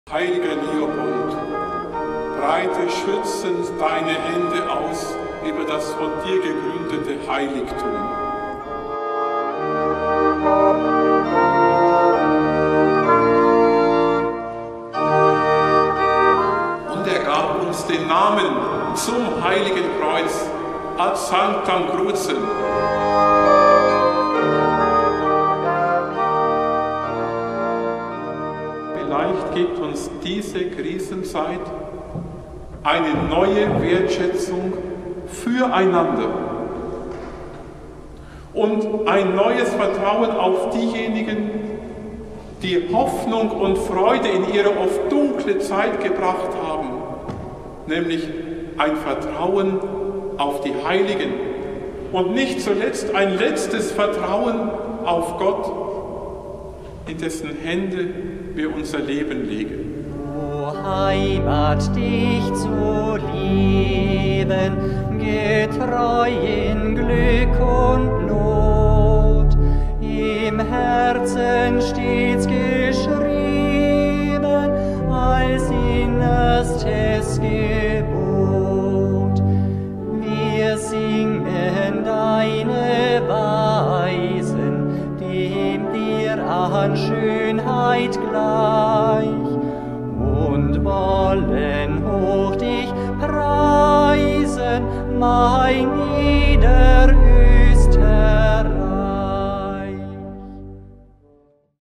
Heiliger Leopold, bitte für uns! Und bitte für unser schönes Österreich!
Dem heiligen Markgrafen Leopold von Österreich verdankt das Kloster Stift Heiligenkreuz seine Existenz. Er hat auf Anregung seines Sohnes, des seligen Otto von Freising, das Stift Heiligenkreuz im Jahr 1133 gestiftet.
Heiliger Leopold, breite schützend deine Hände aus über dein gegründetes Heiligtum!

Videoproduktion: Elisabeth Fürst
Herzlichen Dank an das Studio1133 für die Bereitstellung der Drohnenaufnahme von Stift Heiligenkreuz am Ende des Videos.